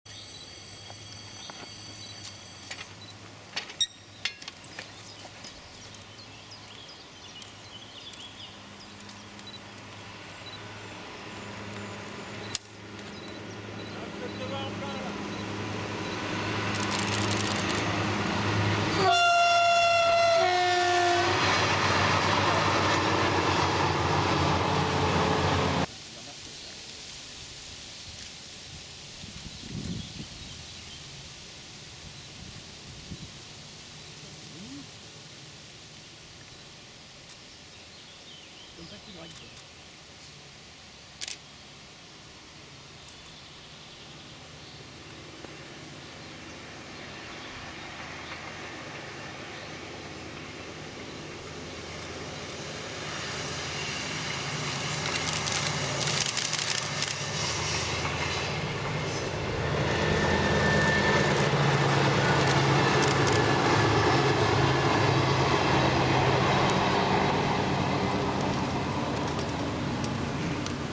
Autorail Festirail 2012 Montluçon
passage à l'écluse sur le canal du Cher des autorails X2800 et X2400 lors du Festirail 2012